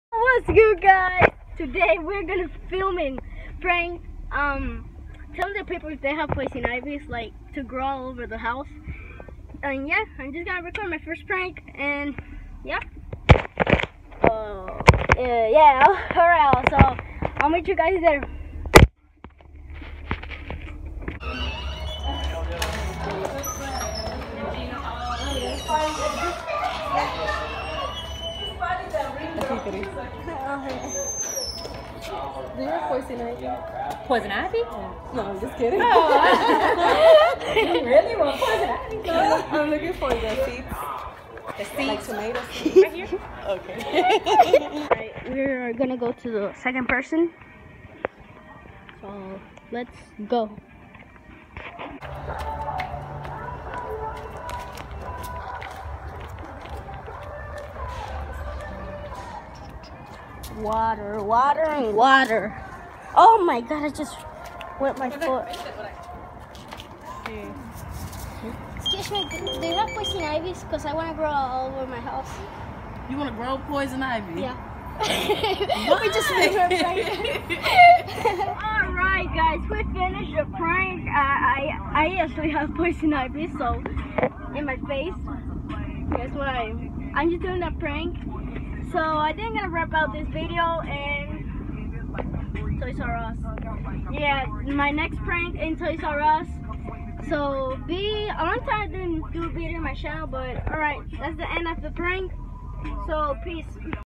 Poison Ivy prank in HOME DEPOT (GONE WRONG!!!)
this video is my first prank in my channel so please subscribe to my channel. cristian molinavlogs so yea and sorry for the audio and y hope you guys have an amazing day .peace